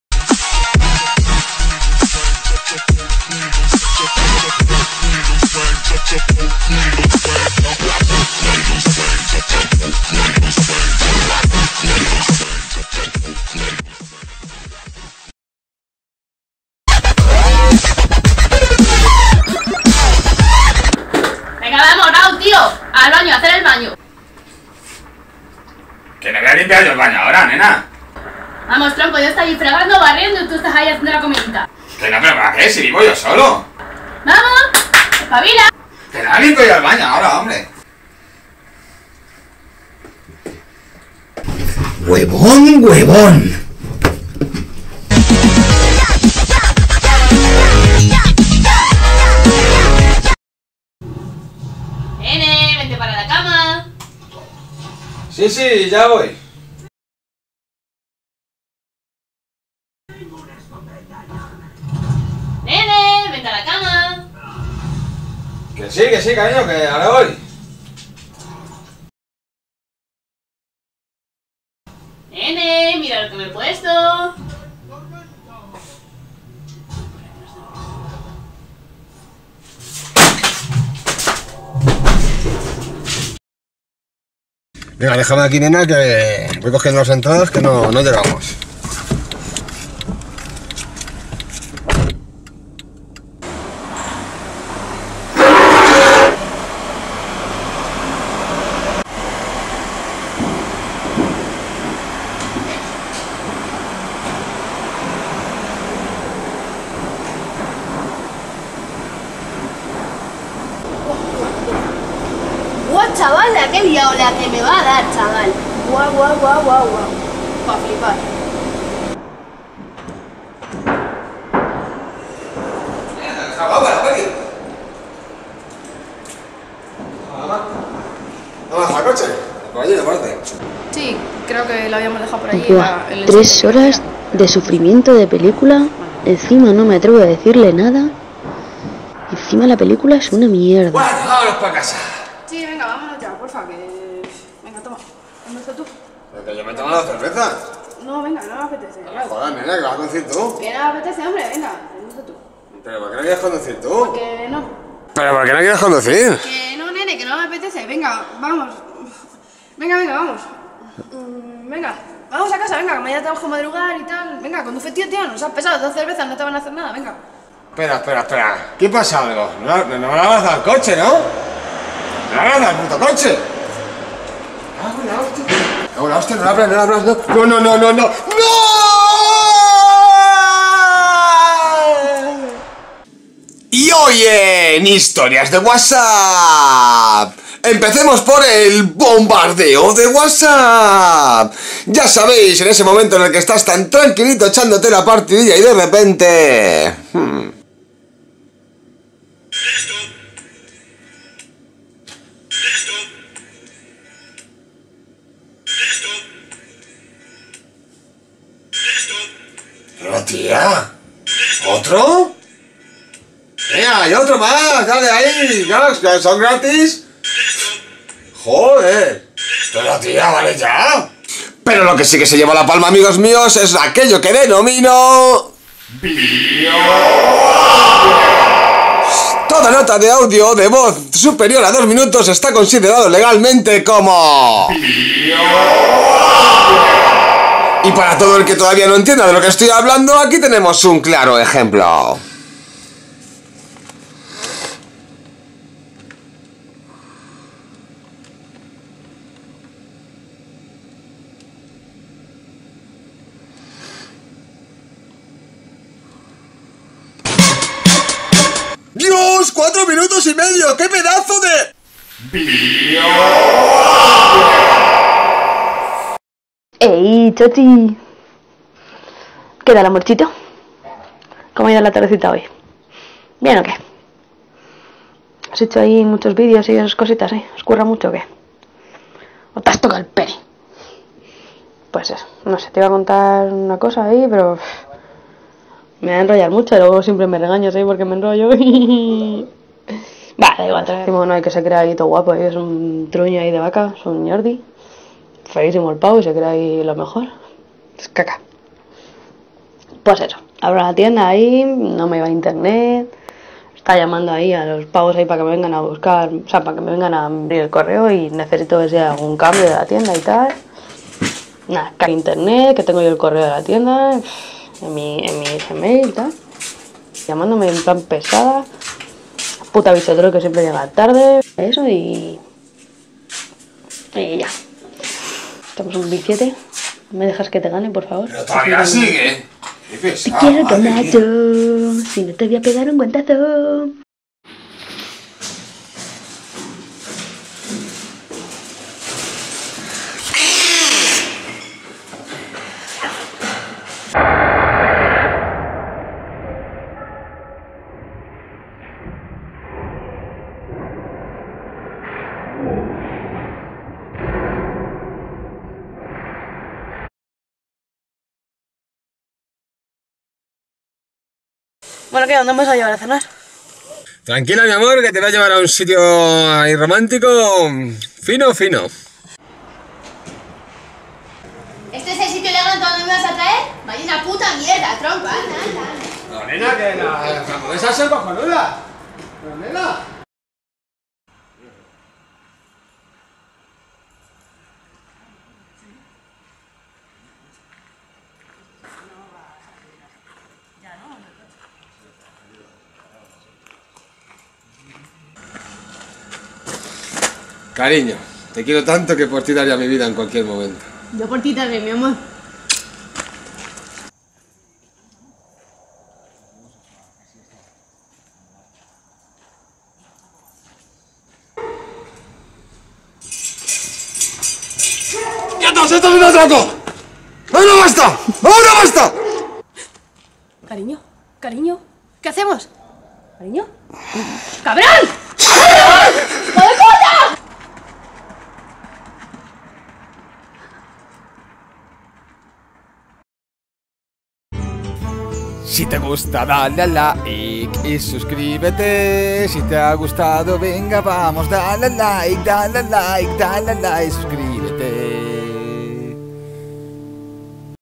Historias de pareja
¿Sufres bombardeo de wasaps?
¿Tu pareja te rayó el coche y no sabia como decírtelo?
¿Sabes lo que es un biblioaudio?
Situaciones reales que pasan en el día a día de la vida de pareja, donde nos encontramos en esos momentos de risas, sustos, alegrías y alguna que otra situación no tan deseada. Seguramente os sintáis identificados en algunas de ellas. Si es así deja tu comentario.